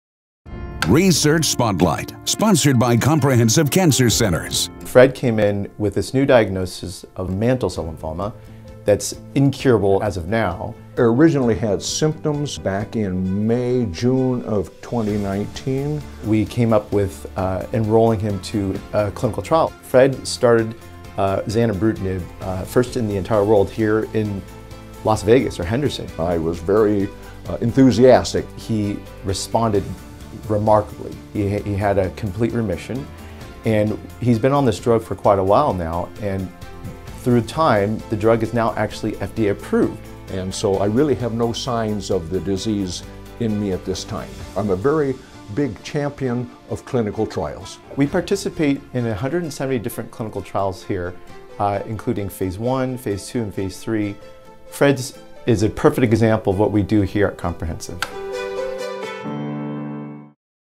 Research Spotlight – Fred
Las Vegas local Fred Warnick recently became the first patient in the world to enroll on a new clinical trial for lymphoma, and it’s happening at Comprehensive Cancer Centers.